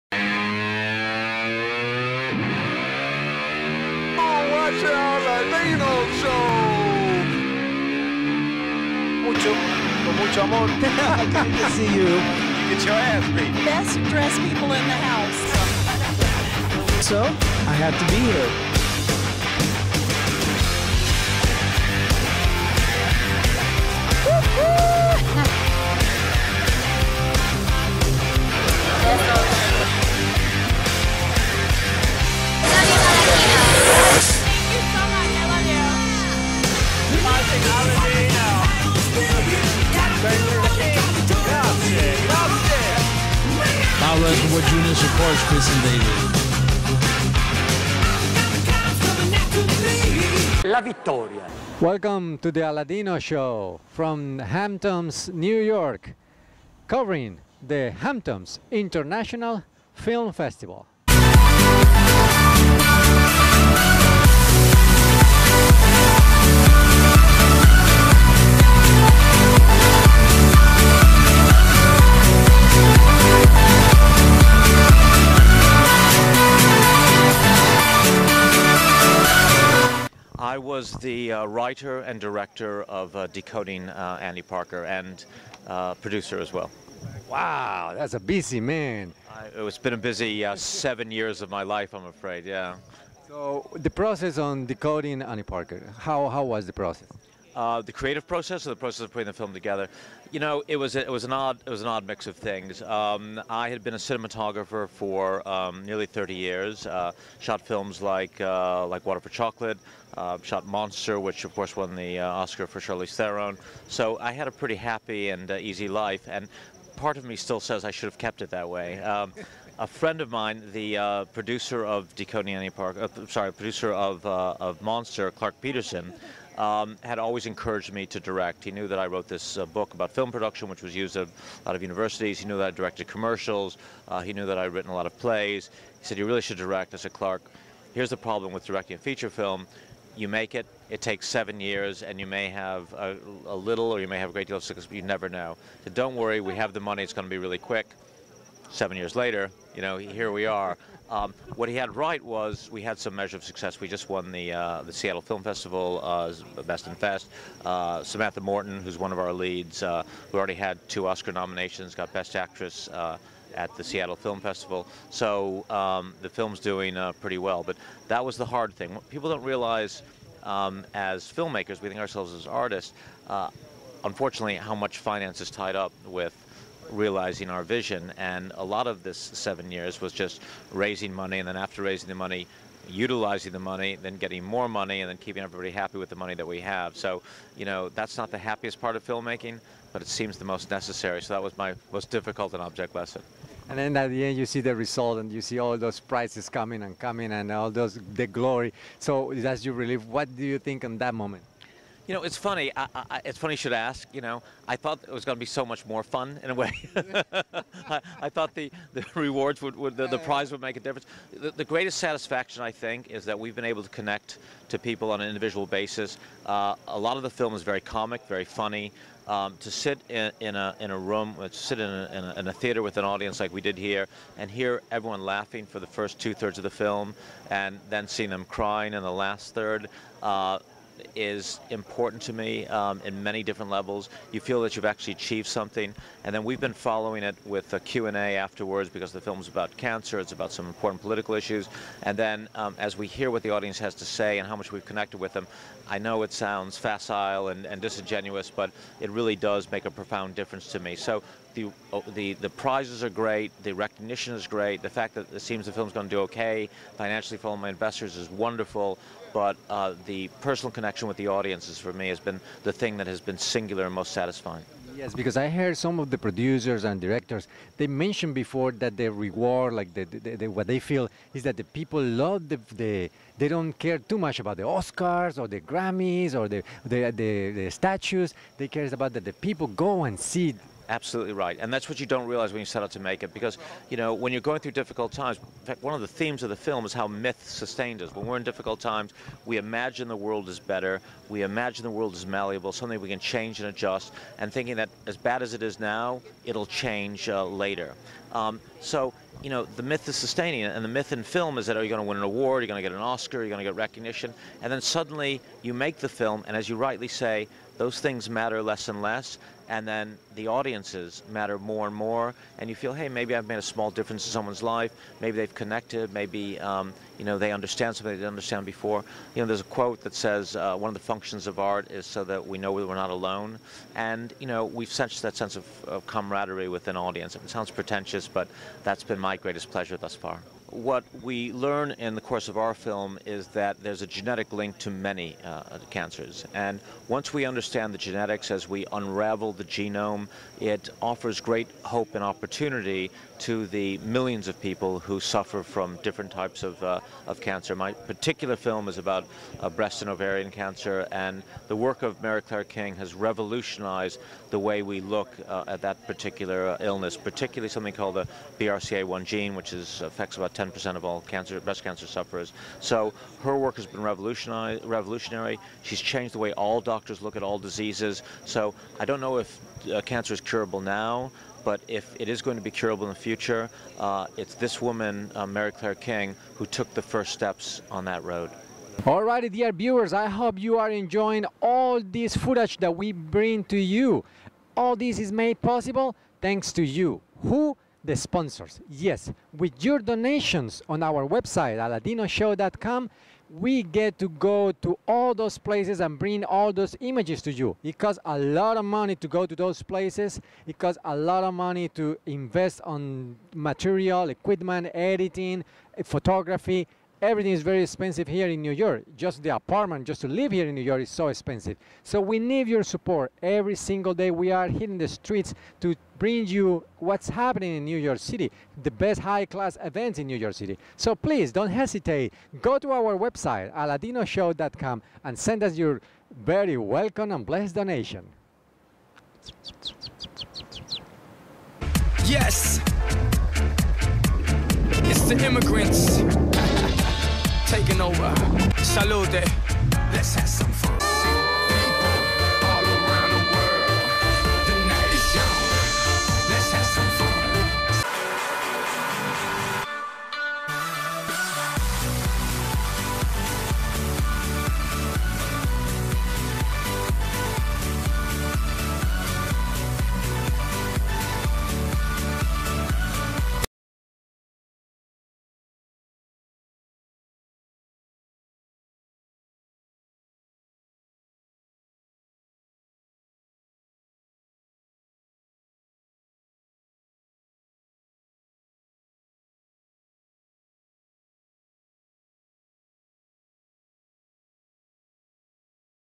Interview with Steven Bernstein
Annie Parker (Samantha Morton) finds our she has breast cancer, a devastating blow to a woman who's already lost her mother and sister to the same disease. Mary-Claire King (Helent Hunt) is a geneticist researching the still unknown link between DNA and cancer in the wake of skeptical colleagues and funding challenges. The journey of these two woman meet in Steven Bernstein's powerful new film. Based on a true story, Bernstein's film catalogues Annie's and Mary-Claire's personal and emotional struggles with delicacy and humor, crafting an inspiring human story at the center of a monumental breakthrough in medical science.

The 2013 Alfred P. Sloan Foundation Feature Film Prize at the Hamptons International Film Festival goes to Steven Bernstein's powerful feature film, DECODING ANNIE PARKER.

Bernstein is an award-winning cinematographer who began his career in the UK in music videos and commercials, winning the Cannes Golden Lion, with a career spanning three decades whose work on groundbreaking projects includes the multi-award winning feature LIKE WATER FOR CHOCOLATE and the Oscar-winning film MONSTER starring Charlize Theron. As an author, his textbook on film making, FILM PRODUCTION, was the largest selling book about film production for over a decade and has been translated into several languages and is still used in universities around the world. He has lectured at many universities in the United States and the United Kingdom. Steven Bernstein begins production as a writer and director on his next film DOMINION about the life and final day of the poet Dylan Thomas.